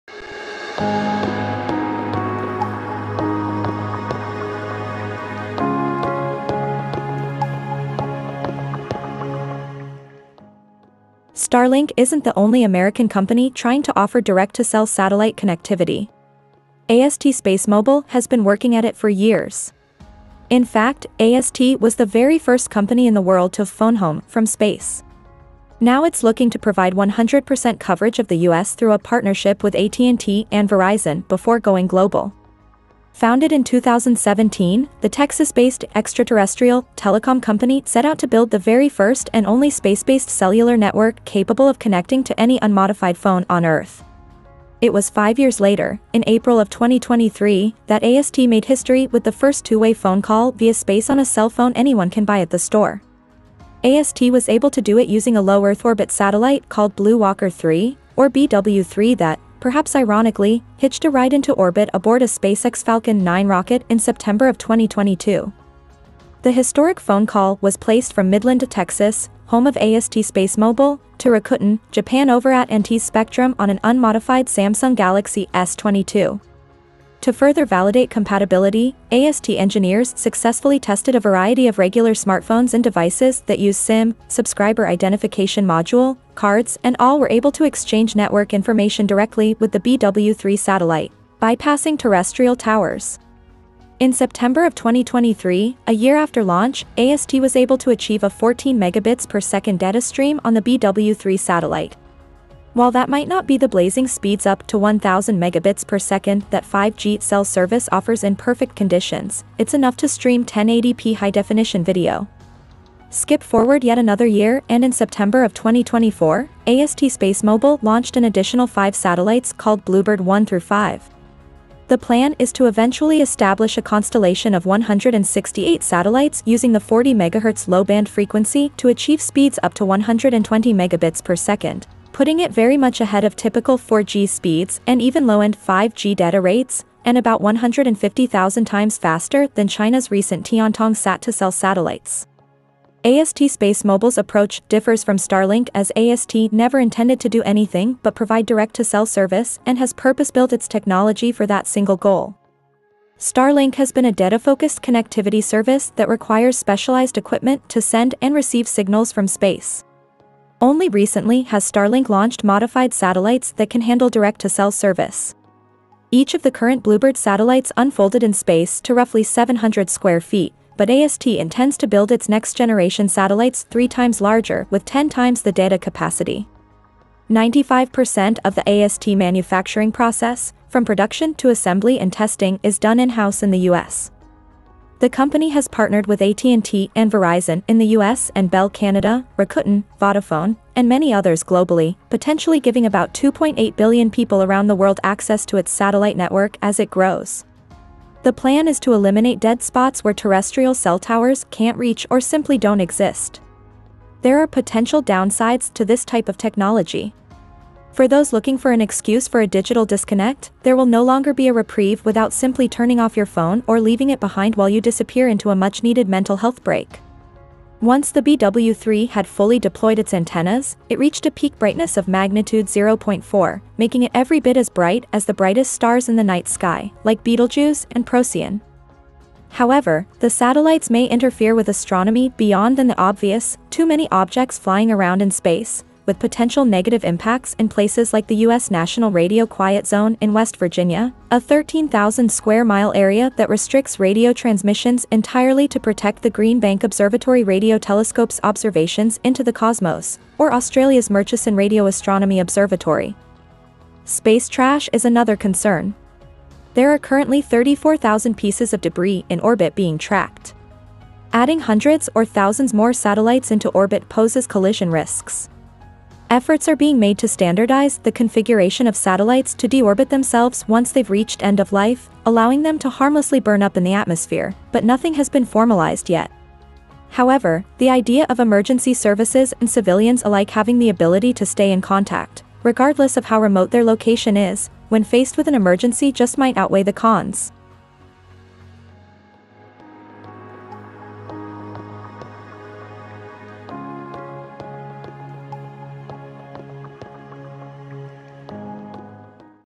This Massive Satellite Beams High Speed Internet Directly to Your Cell Phone from Outer Space
The Texas based extra terrestrial telecom company AST SpaceMobile set out to build the very first and only space based cellular network capable of connecting to any unmodified phone on Earth. It was five years later, AST made history with the first two way phone call via space on a cell phone anyone can buy at the store.

AST SpaceMobile launched an additional five satellites called BlueBird 1 through 5. The plan is to eventually establish a constellation of 168 satellites using the 40 MHz low band frequency to achieve speeds up to 120 Mbps, putting it very much ahead of typical 4G speeds and even low end 5G data rates and about 150,000 times faster than China's recent Tiantong sat-to-cell satellites.